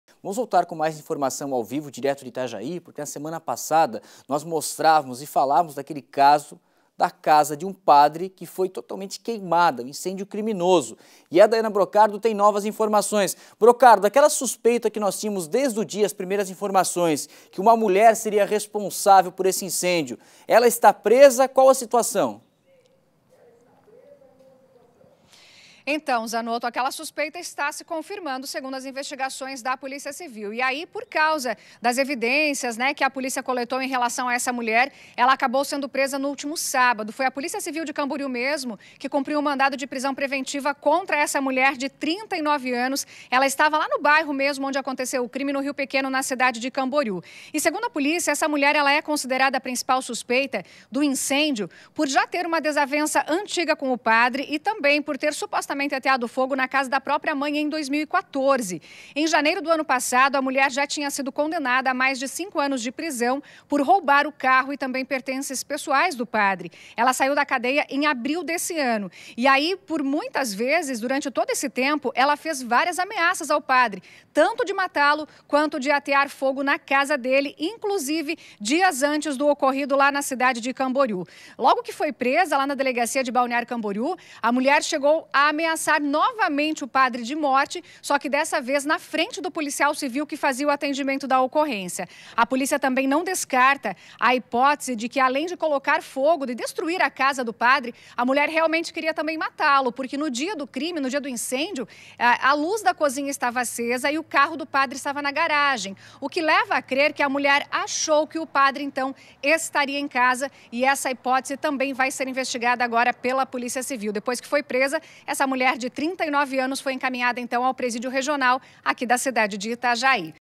Mulher suspeita de incendiar casa de padre é presa em Camboriú
Uma mulher foi presa por suspeita de colocar fogo na casa de um padre em Camboriú. A moça, de 39 anos, ja era a principal suspeita do crime por ter uma antiga desavença com o padre e também por ter ateado fogo na casa da mãe em 2014.